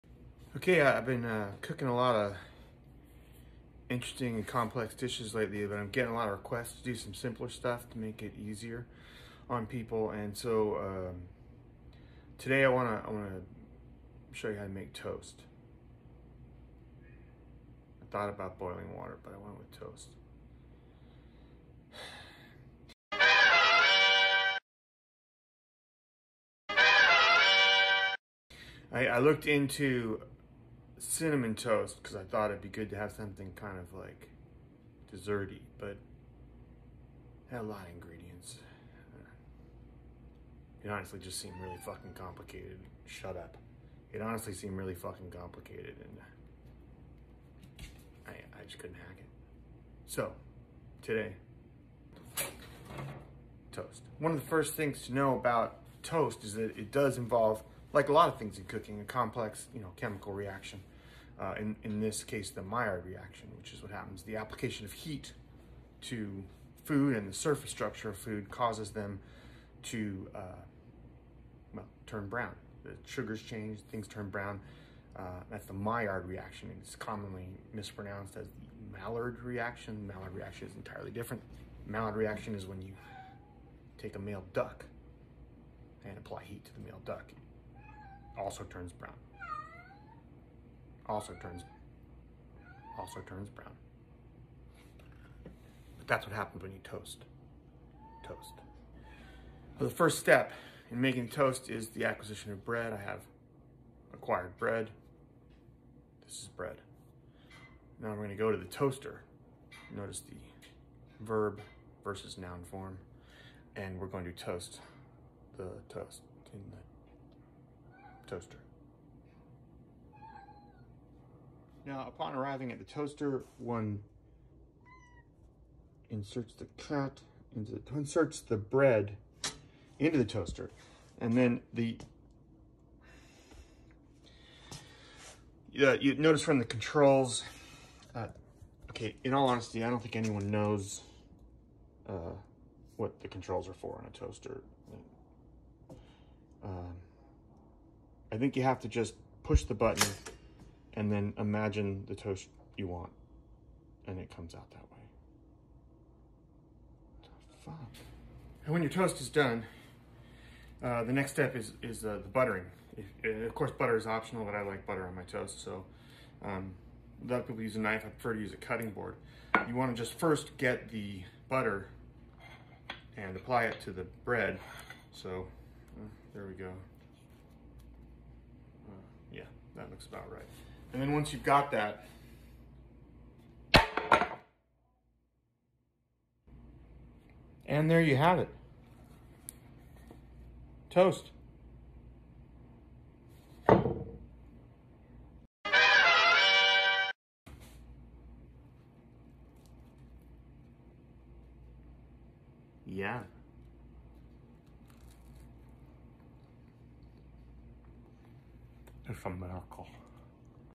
What's Cooking with Adam Duritz? - Toast
With everyone going on this week, I wanted to really concentrate on a recipe for all my vegetarian friends. I considered a wild mushroom soufflé but decided on toast instead. (shrug) With my helpful cooking hacks, it’s not as hard as it looks. Blueberry helped out. Try it. Make it. Feed yourself.